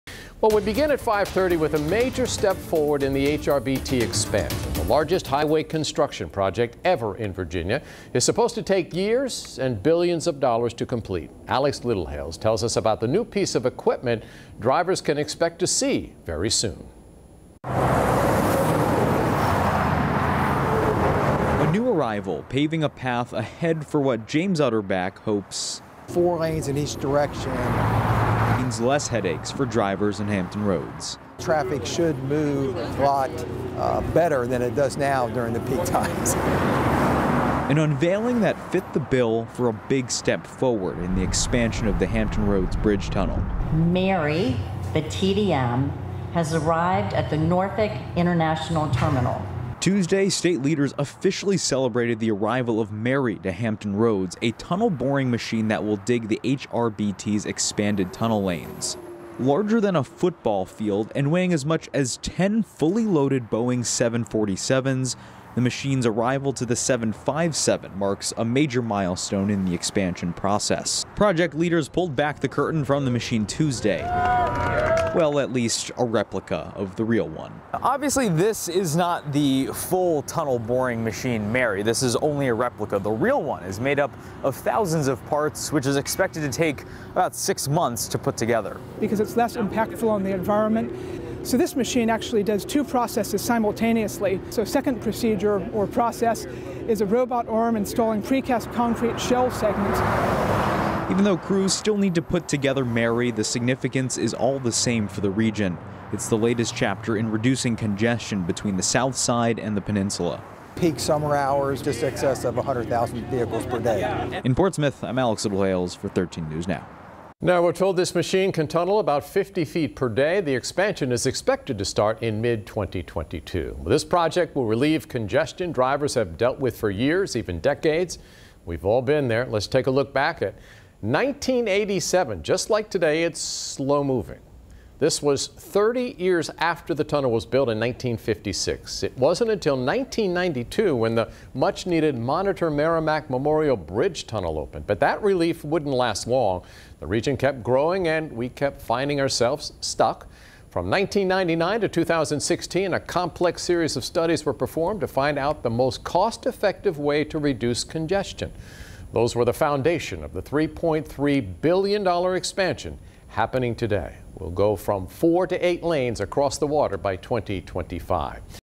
Boring Machine Arrives for HRBT Tunnel Expansion
The machine will be able to tunnel about 50 feet a day.